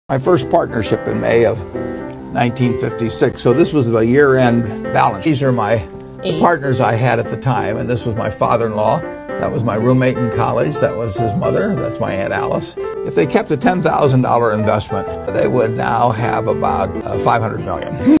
😳 Warren Buffet's relations would have made $500,000,000 from just $10,000 investment
😳 Warren Buffet's relations would have made $500,000,000 from just $10,000 investment . Warren Buffet relations would have earned millions of dollars if they had just invested 10k dollars.